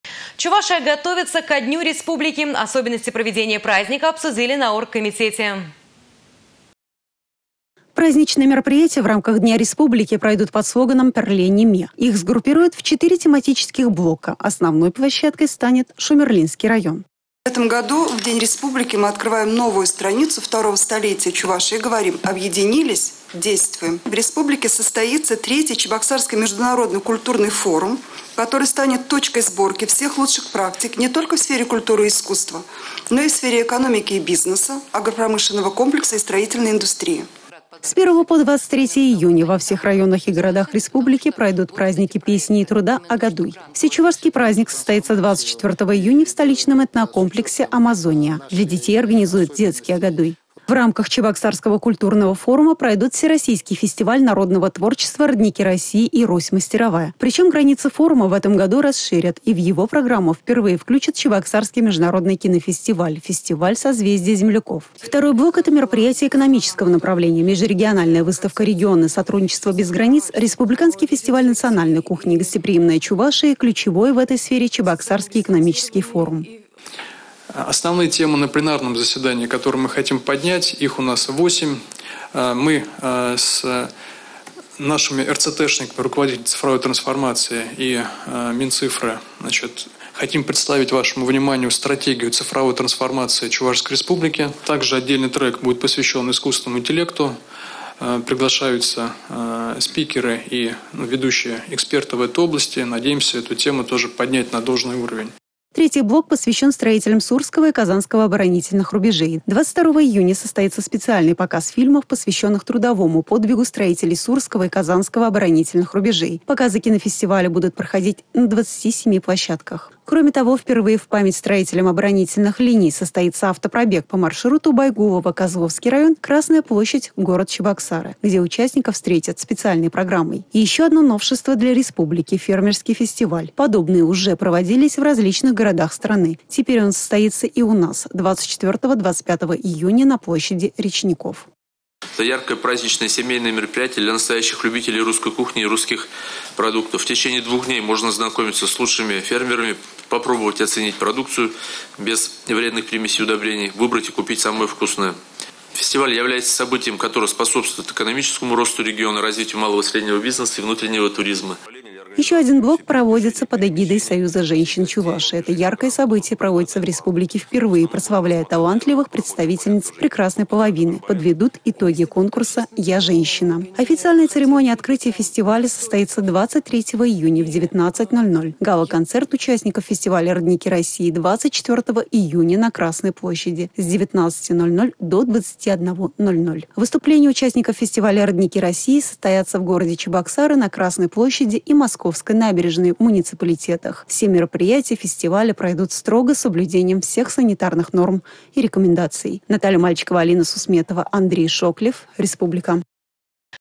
Чувашия готовится к Дню Республики
Чувашия готовится к Дню Республики. Особенности проведения праздника обсудили на оргкомитете.